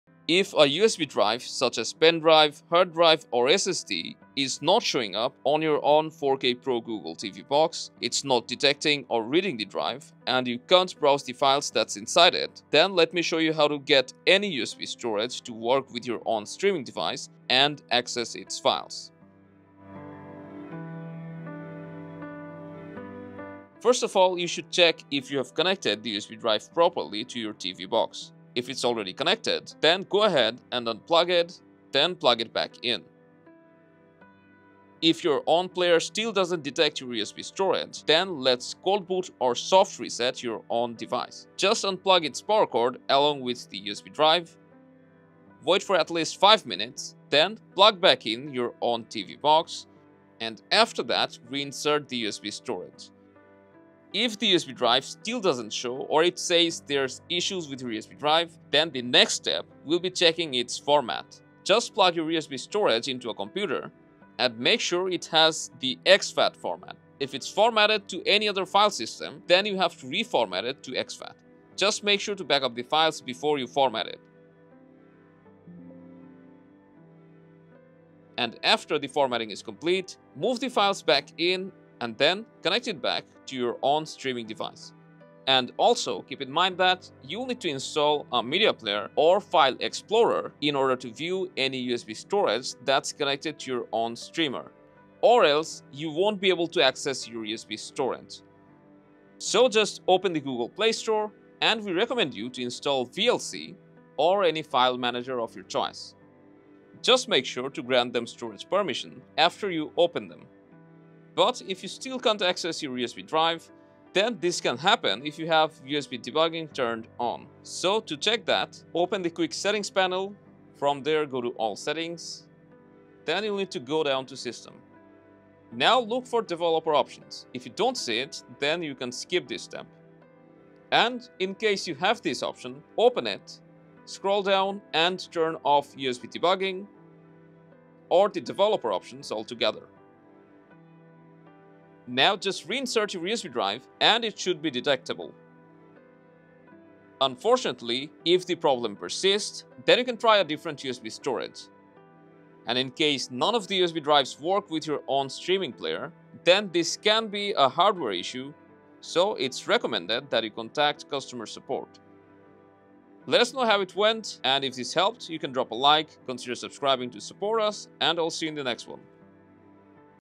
Onn. 4K Pro Box Won't Recognize/Read USB Flash Drive!
ONN 4K Pro Google TV Streamer not recognizing your external USB Storage, like a Flash Drive, Pen drive, SSD, HDD, Memory Stick, Thumb Drive, and More? Unable to play pictures, videos, music, and more media files on your Walmart Onn 4K Pro Player from a USB Drive and struggling with various problems mentioned below:

• Unable to read USB Drive
• Issues with your USB Drive
• Not detecting USB Drive
• USB Drive not showing up
• USB Drive not supported
• USB Drive not showing media files
• Can’t play media files from USB Drive

The Fix369 Team brought you 5 troubleshooting methods to fix the USB External Drive not working on the ONN 4K Pro Box with Googe TV swiftly.

0:00 Intro: ONN TV Box not reading USB Drive
0:23 Solution 1: Basic Troubleshooting
0:52 Solution 2: USB Drive Format
1:24 Solution 3: Install a Media Player
1:50 Solution 4: Turn Off USB Debugging
2:25 Solution 5: Use a Different USB Drive/ Contact Support